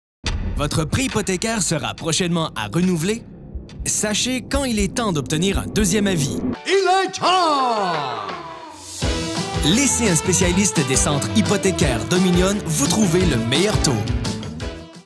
Renouveler n’a pas à être compliqué! (Fr)
Votre prêt hypothécaire est à renouveler? Il est temps de discuter de vos options avec un courtier des Centres Hypothécaires Dominion
De meilleurs taux, un meilleur service, de meilleurs résultats!

Trouvez un spécialiste hypothécaire près de chez vous.